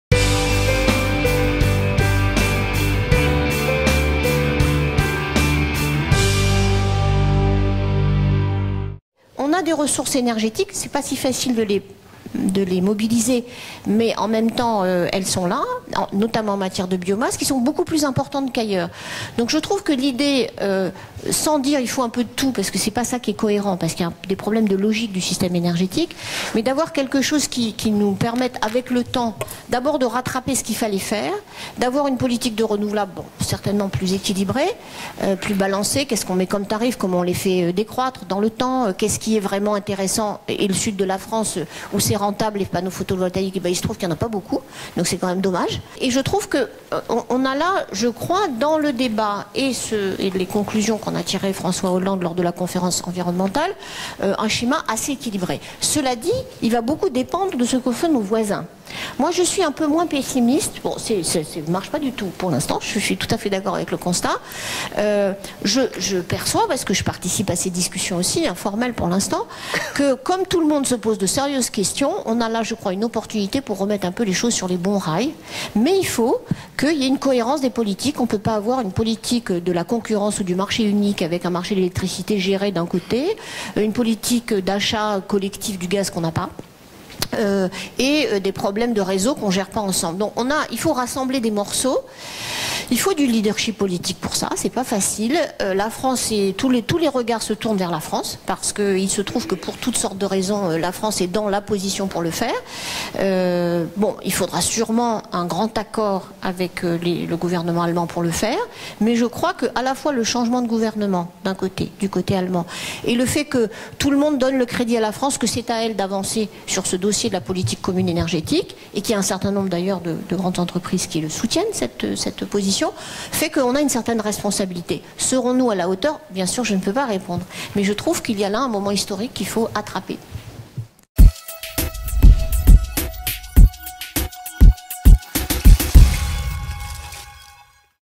Laurence Tubiana : « C'est à la France d'avancer sur la politique énergétique commune »
Les acteurs français de la transition énergétique sont nombreux, mais rien n'avancera vraiment sans décision à l'échelle européenne. Sur ce dossier aussi, l'Allemagne est incontournable, estime Laurence Tubiana, directrice de l'Institut du développement durable et des relations internationales [...]